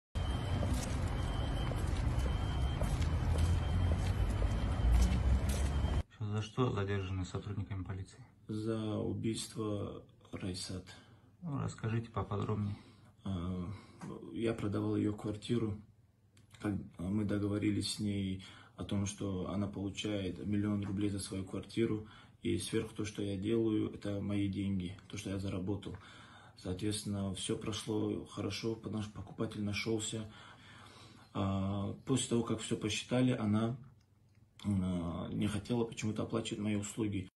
Задержан убийца девцшки в Махачкале
Сотрудники Уголовного Розыска МВД Дагестана установили личность 25-летнего жителя Махачкалы, подозреваемого в убийстве, и задержали его.

Подозреваемый рассказал, что был риэлтором и помогал погибшей продать квартиру. Узнав, что женщина уже заключила сделку, он ожидал получить ранее оговорённую сумму.

5 июня, когда она получила оставшиеся деньги - 440000 рублей, они встретились, но беседа получилась конфликтной и подозреваемый нанёс девушке удар по голове.

У подозреваемого возник умысел совершить убийство с целью завладения деньгами потерпевшей.

На своём авто, он вывез женщину в бессознательном состоянии в безлюдное место и задушил.

Затем, на территории заброшенного полигона, на окраине селения Аданак Карабудахкентского района, облил тело растворителем и сжёг, чтобы скрыть содеянное.

Собранный полицейскими материал передан по подследственности.
СУ СК России по РД возбуждено уголовное дело по ч.2 ст.105 (убийство) и ч.4 ст.162 (разбой) УК РФ.

МВД по Республике Дагестан